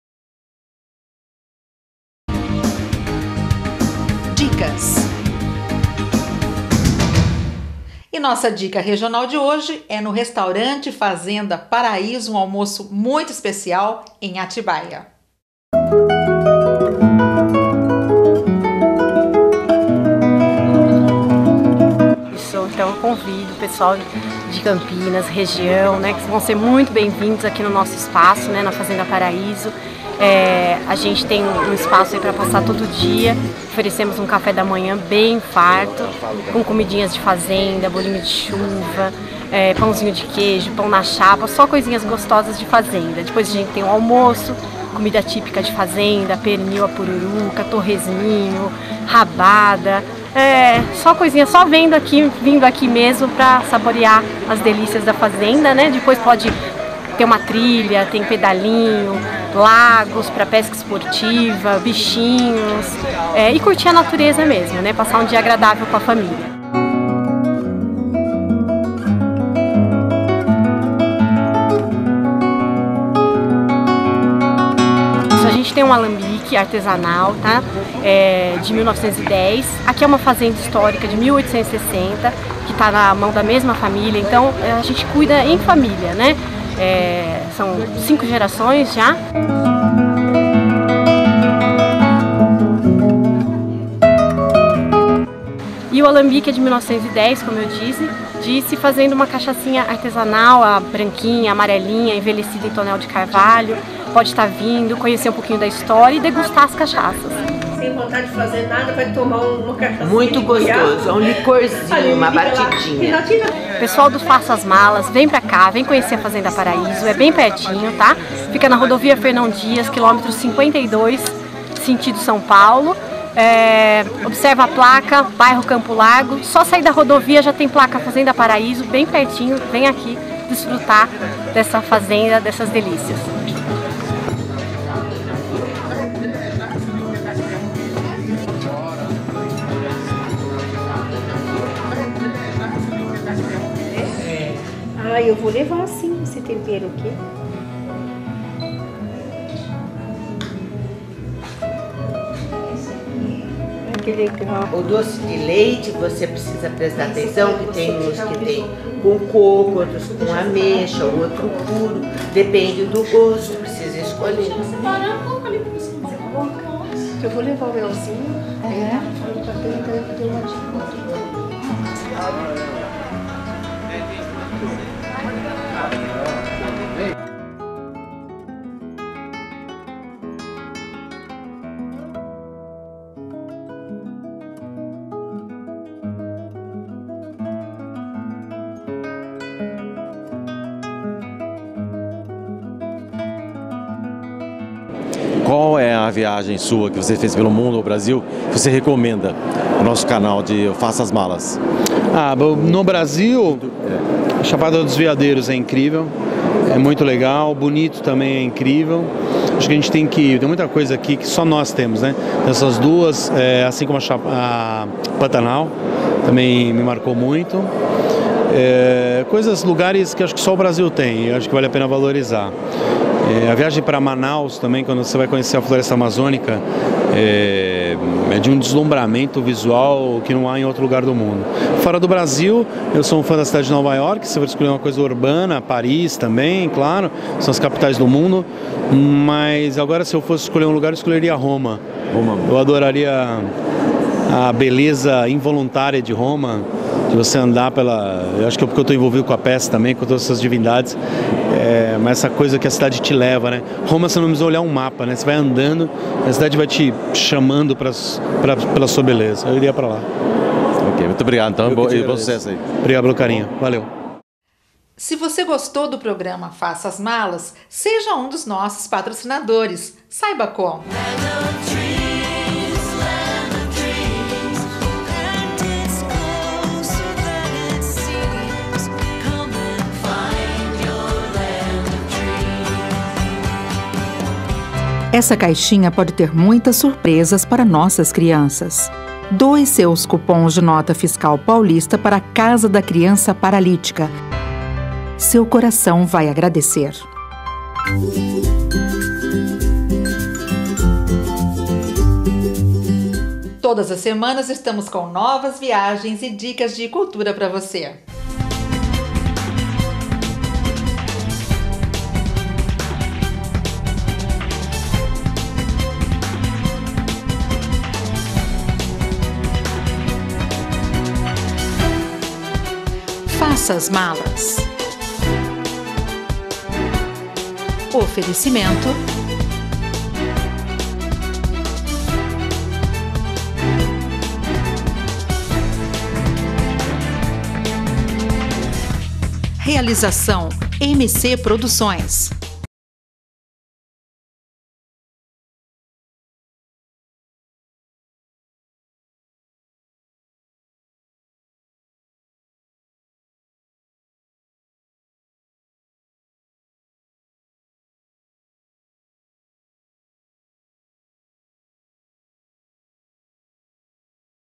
RESTAURANTE FAZENDA PARAISO
Em Atibaia , um local perfeito para se passar ums horas agradáveis e ainda ter uma refeição espetacular